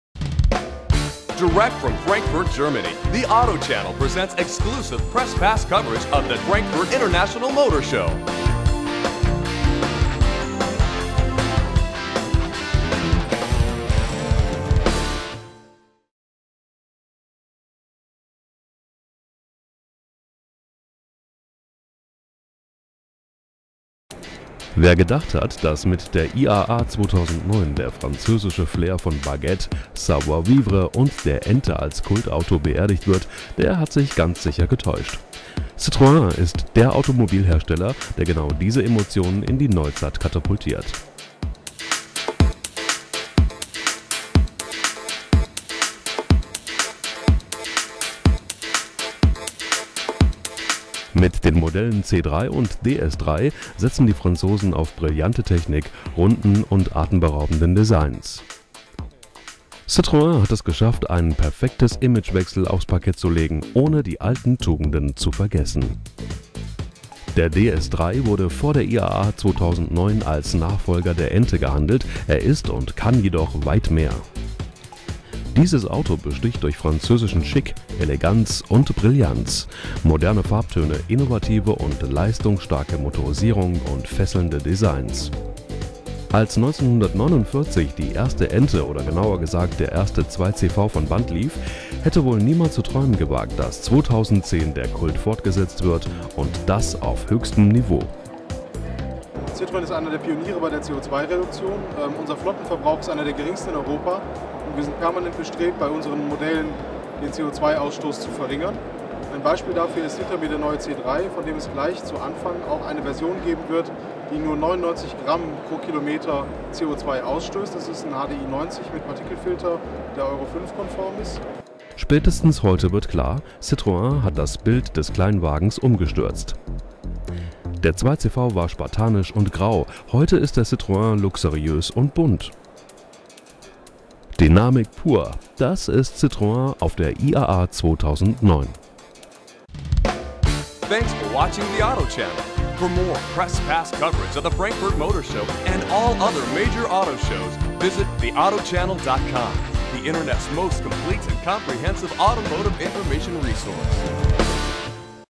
Citroen at the 2009 Frankfurt Motor Show
In German, but the beauty of the cars and girls speak a universal language. You can always read the text story in English on TheAutoChannel.com.